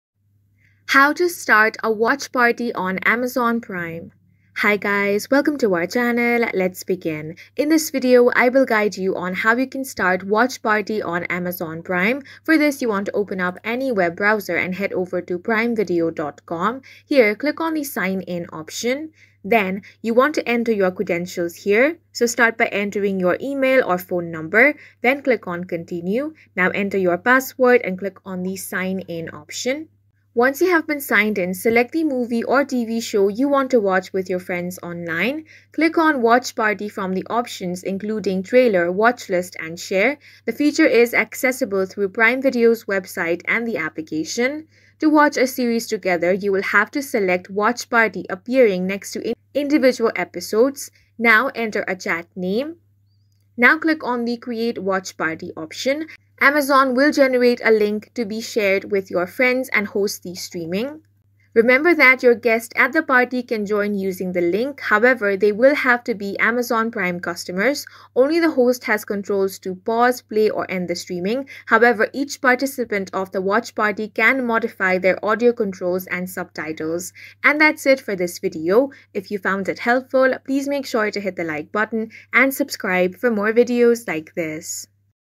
How to Start Watch Party on Amazon Prime (2025) | Amazon Tutorial
Want to host your favorite movies and shows with friends remotely on Amazon Prime? In this guide, we’ll walk you through the steps to start a Watch Party on Amazon Prime Video in 2025. Whether you’re hosting a virtual movie night or binge-watching with friends, this step-by-step tutorial will help you set up and enjoy the feature effortlessly.

Timestamps:
00:00 – How to Access the Watch Party Feature on Amazon Prime
00:10 – Step-by-Step Guide to Starting a Watch Party